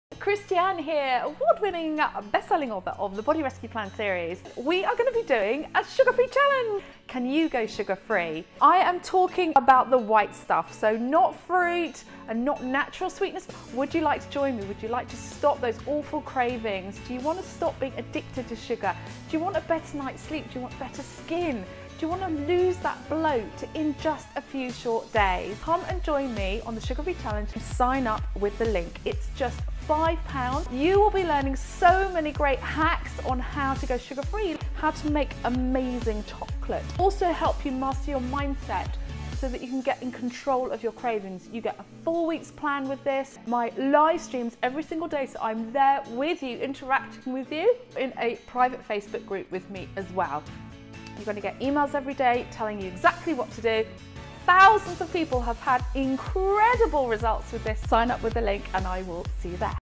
Sugar Free Challenge April 2022
Join my Sugar Free Challenge! Monday 25th April 2022

Imagine NEVER having to battle your sugar cravings again!
The Body Rescue Sugar Free Challenge is a 5 day Body and Mind makeover, so you can take back your power ( even when nothing else has worked )

Do you feel like your relationship with SUGAR has been a bit like ground hog day ? Maybe…

➡️ You get super excited to get fit and eat better, only to fall back into your old patterns again.
➡️ You're tired of opening the wardrobe and feeling like nothing fits.
➡️ You try to eat well but it's like chocolate and biscuits keep calling your name from the cupboard- it feels like you need that comfort right now- especially.